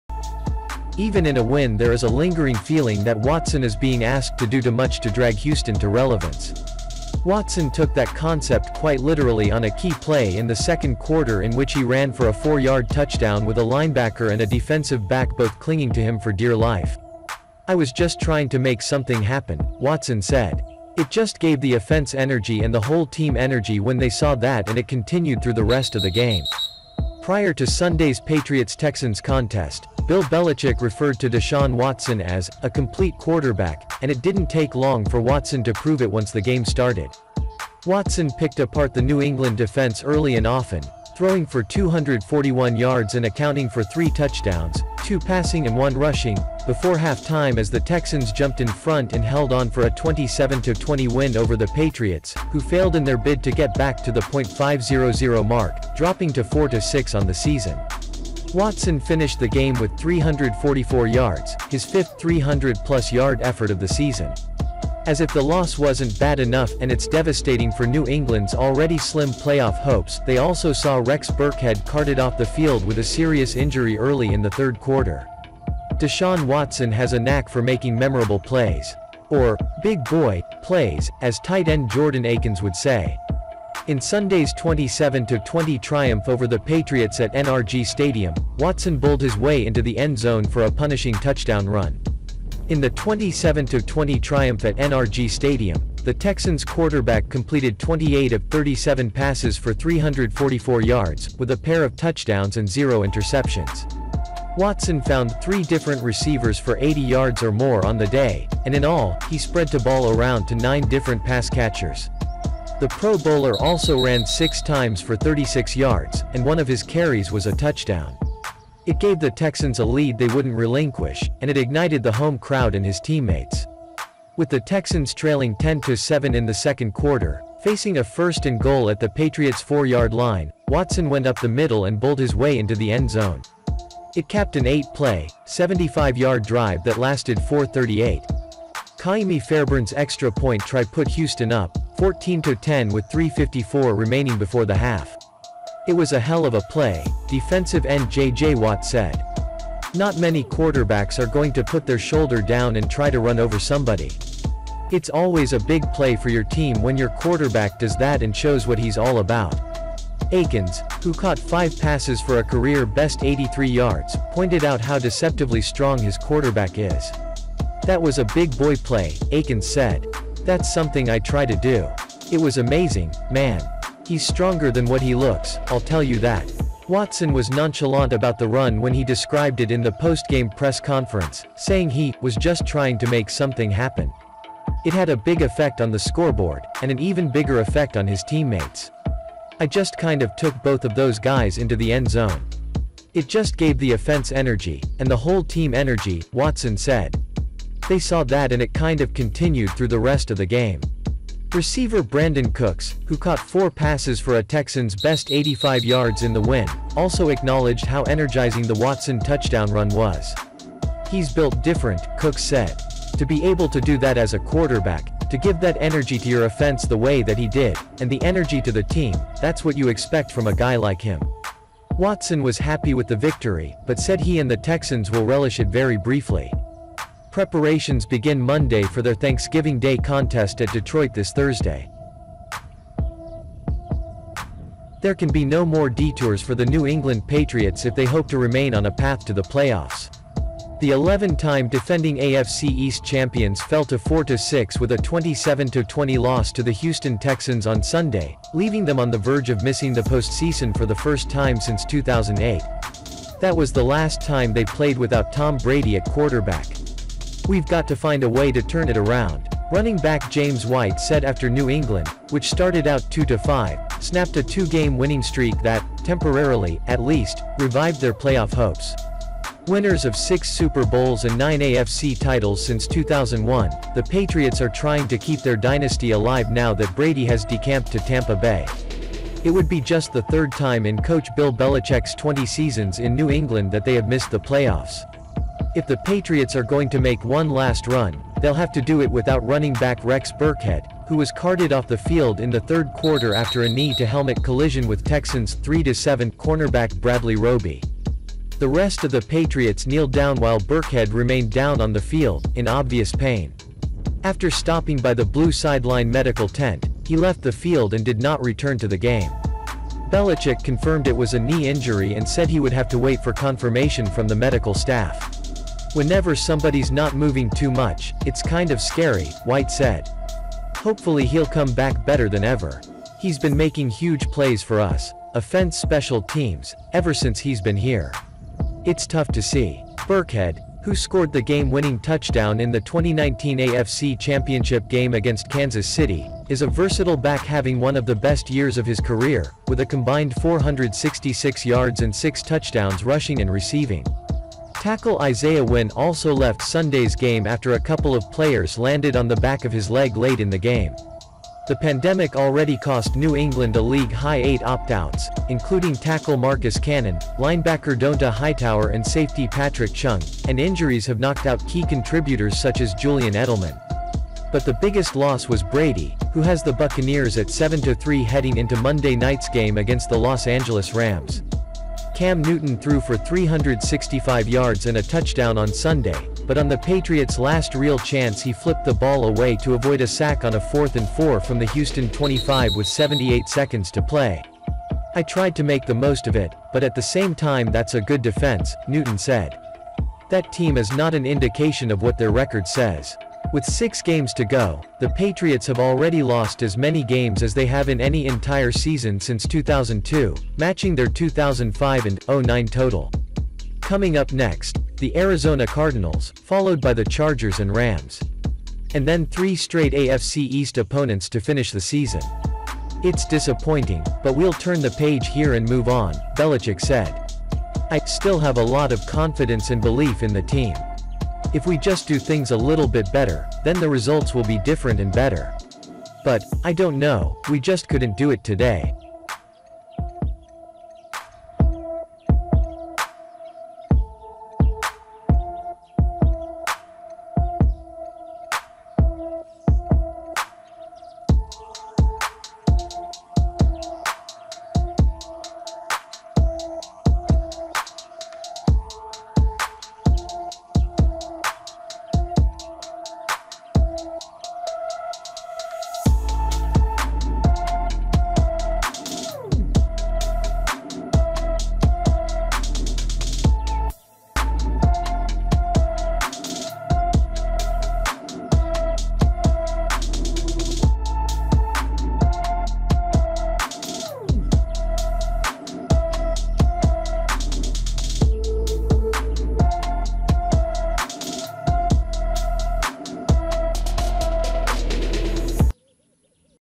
Top Passer: Deshaun Watson EASY take the Texans to Victory - NFL Week 11 Result
Top Passer: Deshaun Watson EASY take the Texans to Victory - NFL Week 11 Result. Even in a win there is a lingering feeling that Watson is being asked to do to much to drag Houston to relevance.
 Watson took that concept quite literally on a key play in the second quarter in which he ran for a 4-yard touchdown with a linebacker and a defensive back both clinging to him for dear life.
“I was just trying to make something happen,” Watson said. “It just gave the offense energy and the whole team energy when they saw that and it continued through the rest of the game.”